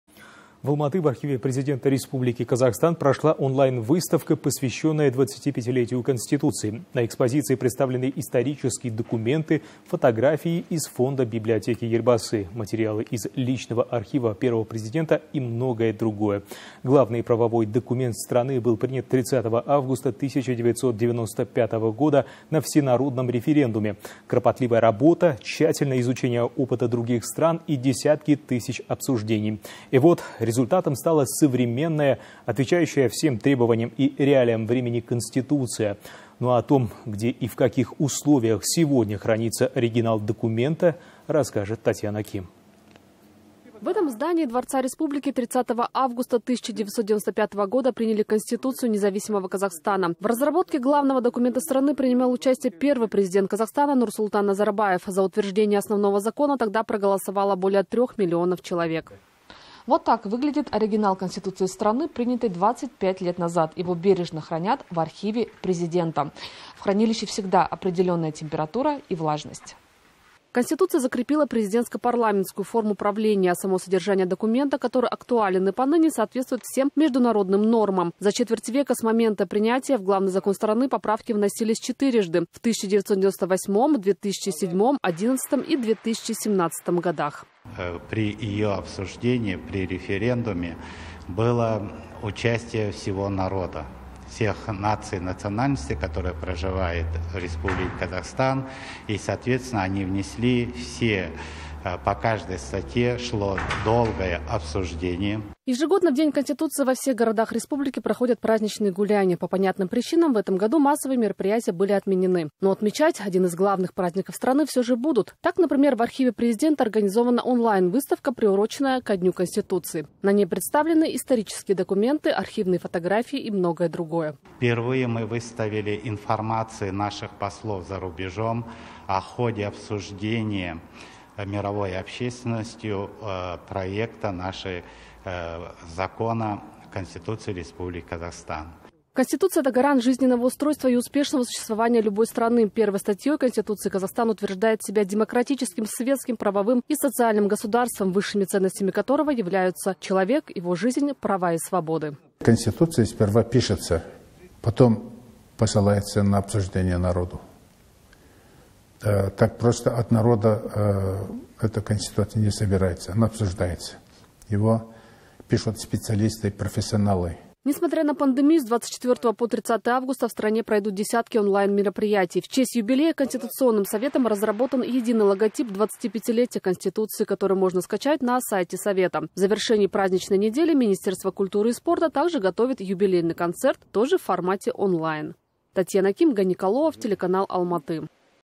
В Архиве Президента провели онлайн-выставку, посвященную 25-летию Конституции (28.08.20)
Архив президента провел онлайн-выставку, посвященную 25-летию Конституции. На экспозиции представлены исторические документы, десятки исторических фотографий из фонда Библиотеки Елбасы, материалы из личного архива Первого Президента и многое другое, передает корреспондент Almaty.tv.

Главный правовой документ страны принят 30 августа 1995 года на Всенародном референдуме. Кропотливая работа, тщательное изучение опыта других стран и десятки тысяч обсуждений - результатом стала современная, отвечающая всем требованиям и реалиям времени Конституция.

В этом здании Дворца Республики 30 августа 1995 года приняли Конституцию Независимого Казахстана. В разработке Главного документа страны принимал участие Первый Президент страны - Елбасы Нурсултан Назарбаев. За утверждение основного закона тогда проголосовало более трех миллионов человек.